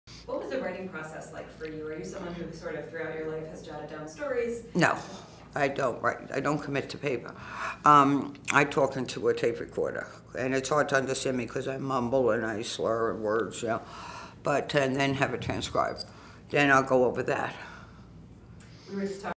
INTERVIEW: Penny Marshall on how she doesn't commit to pa...
INTERVIEW: Penny Marshall on how she doesn't commit to paper, on how she talks into a recorder, uses a transcriber at 'My Mother Was Nuts' Junket - Penny Marshall at IDPR Offices on September 19, 2012 in New York, New York